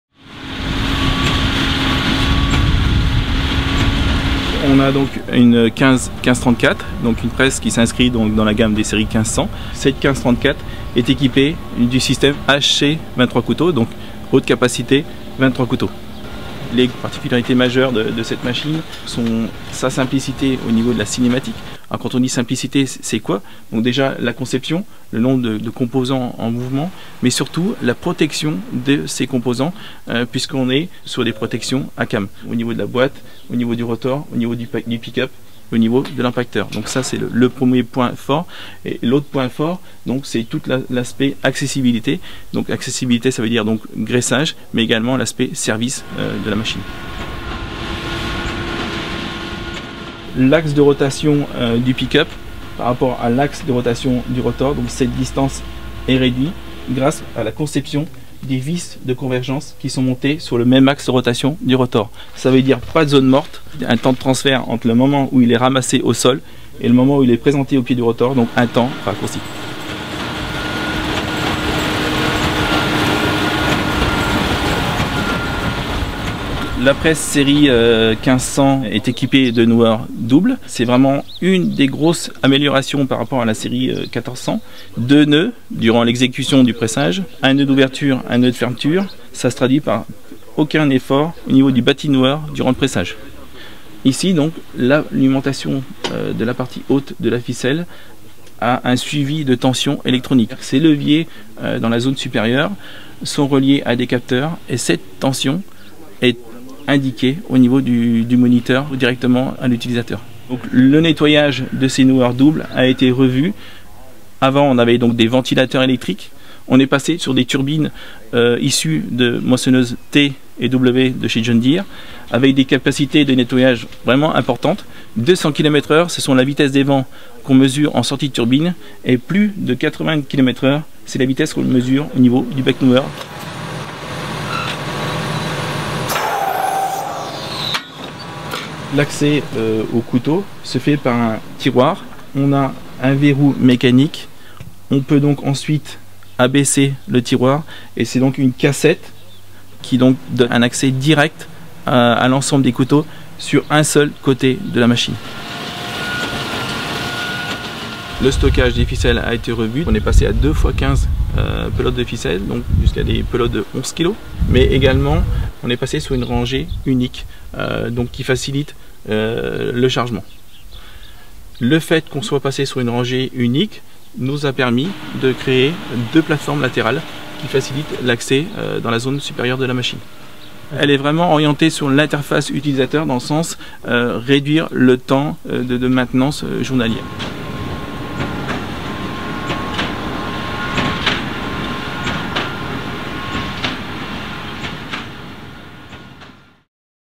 Presse : John Deere montre sa densité
10 concessionnaires ont présenté la nouvelle presse haute densité de John Deere. Sur le secteur des Établissements Lebaudy, la machine a réalisé plus de 2 400 bottes. L'occasion de montrer la machine aux futurs clients et de former les distributeurs à une nouvelle technologie.